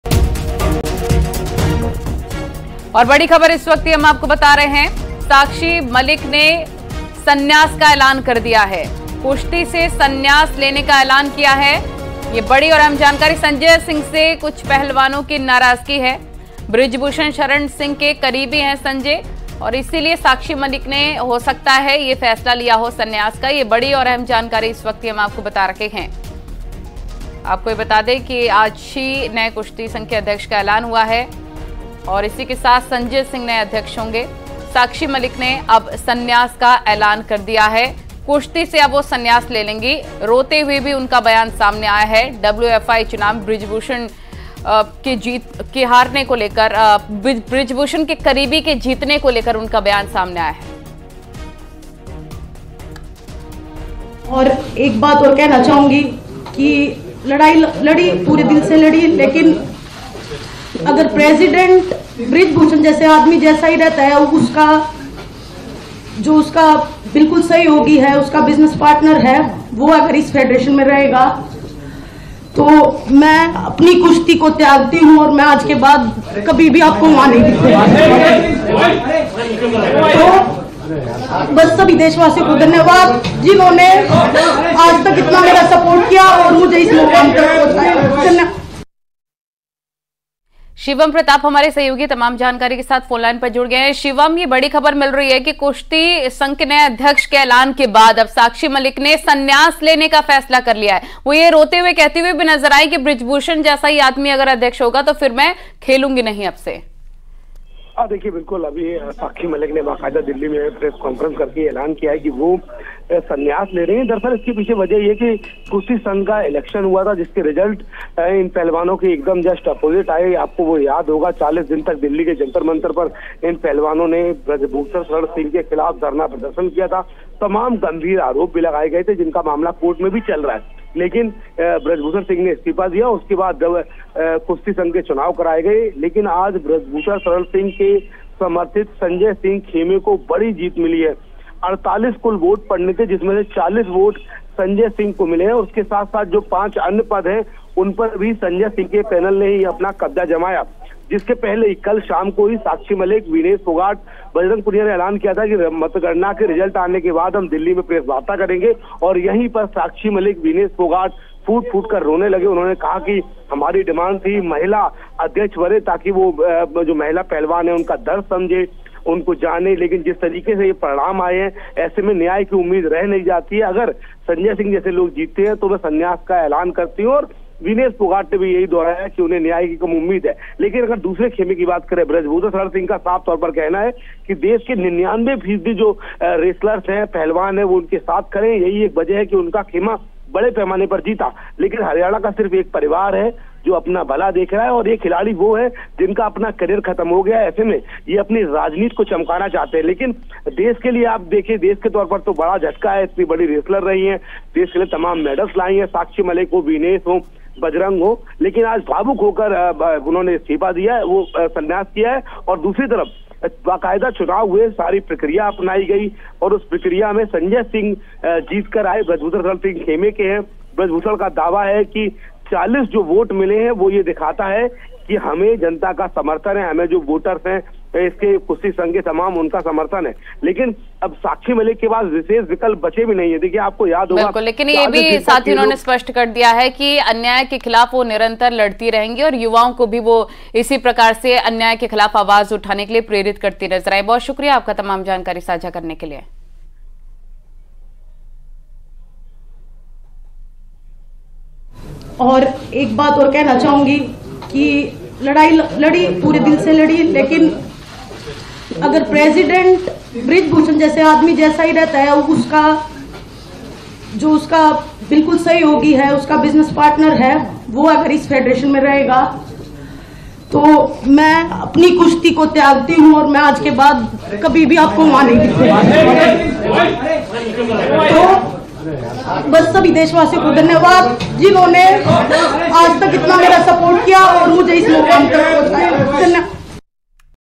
Sanjay Singh के अध्यक्ष बनने से नाराज साक्षी मलिक ने किया बड़ा ऐलान | Sakshi Malik | Breaking News
रेसलिंग फेडरेशन ऑफ इंडिया के नए चीफ संजय सिंह चुने गए हैं। इससे पहले WFI के अध्यक्ष बृजभूषण शरण सिंह थे। लेकिन साक्षी मलिक, विनेश फोगाट और बजरंग पूनिया सहित पहलवानों ने बृजभूषण सिंह पर यौन आरोप लगाए थे। इसी वजह से वह चुनाव नहीं लड़े थे।